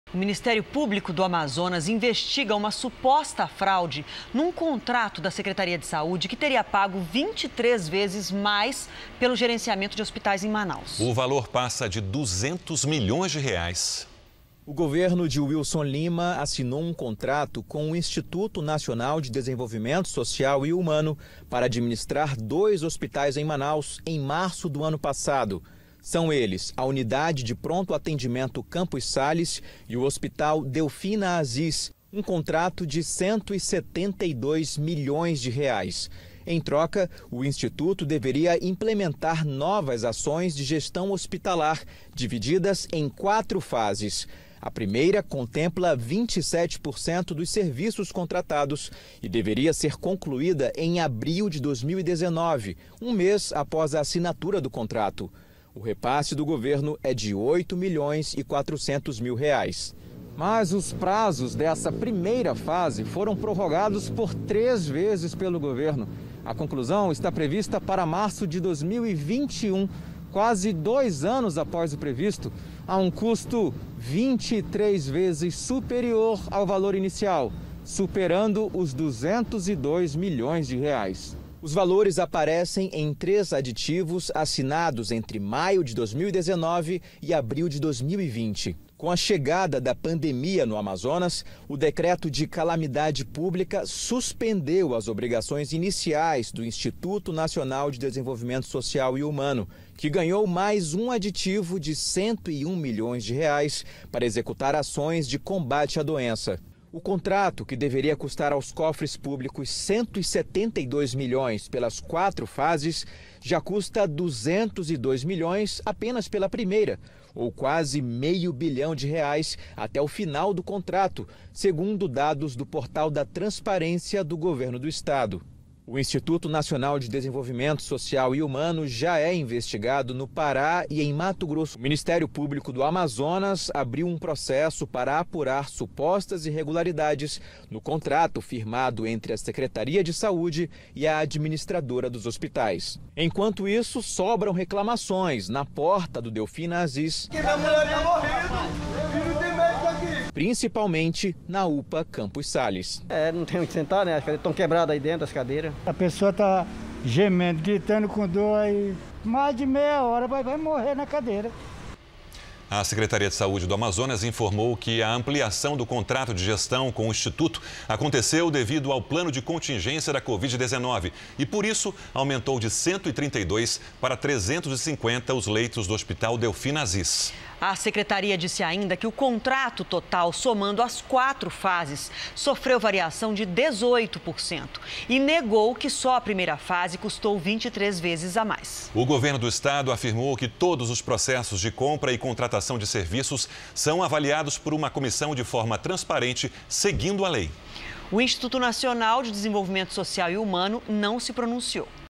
MP do Amazonas investiga suposta fraude em contrato da Secretaria de Saúde
O Ministério Público do Amazonas investiga uma suposta fraude num contrato da Secretaria de Saúde que teria pago 23 vezes a mais pelo gerenciamento de hospitais em Manaus. O valor ultrapassa os R$ 200 milhões.